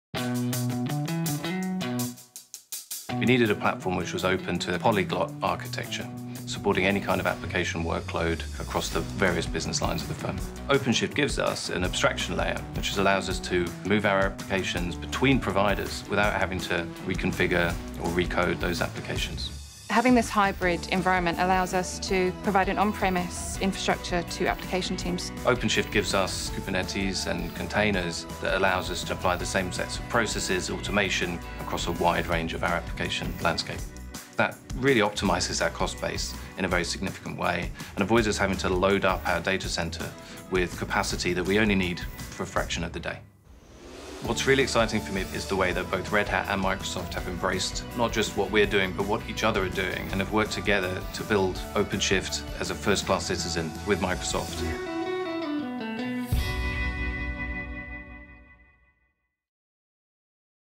Red Hat and Microsoft help Deutsche Bank realize their vision for Everything-as-a-Service
Learn how Deutsche Bank reduced complexity and cost by augmenting their homegrown Platform-as-a-Service with Red Hat OpenShift and Microsoft Azure. This hybrid environment allows Deutsche Bank to provide an on-premise infrastructure to application teams.